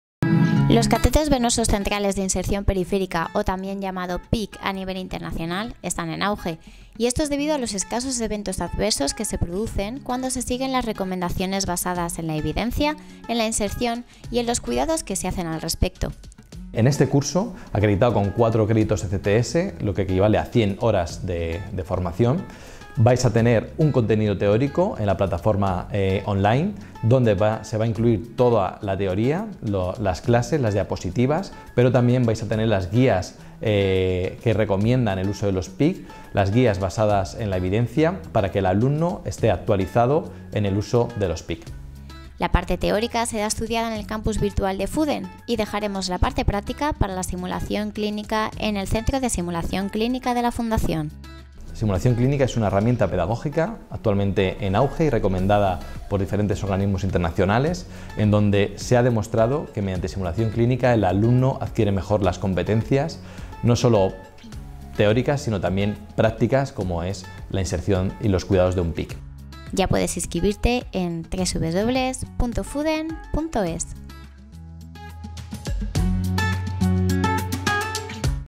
Curso de Inserción, manejo y cuidados de PICC ecoguiados - Fuden TV
En FUDEN hemos diseñado el curso de Inserción, manejo y cuidados de PICC ecoguiados: 100 horas de formación semipresencial destinada a los profesionales de la Enfermería. ¡Infórmate!